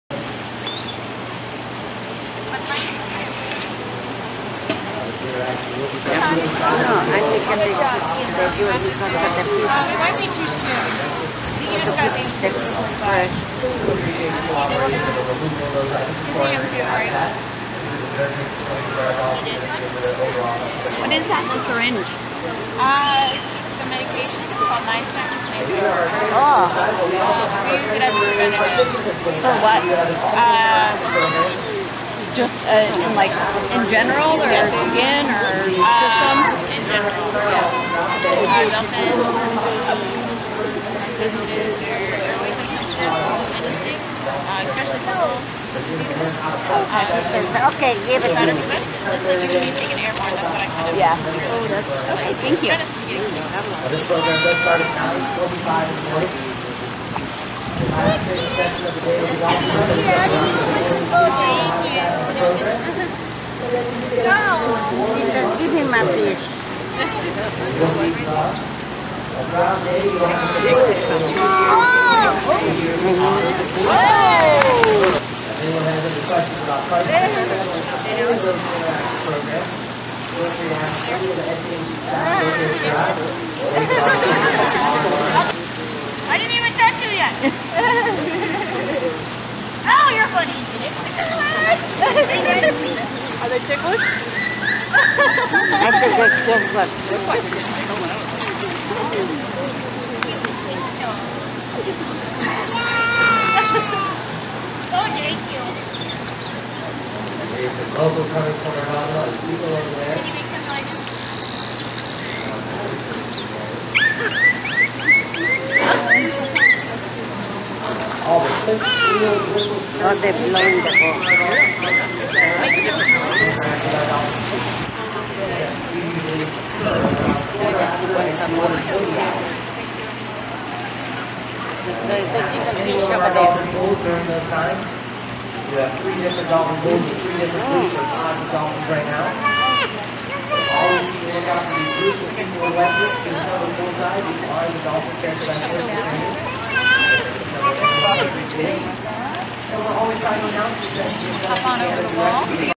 Nystatin and the Mirage Dolphins
This video was recorded in the beginning of March 2015 and shows the dolphin trainers giving Maverick an antifungal called Nystatin.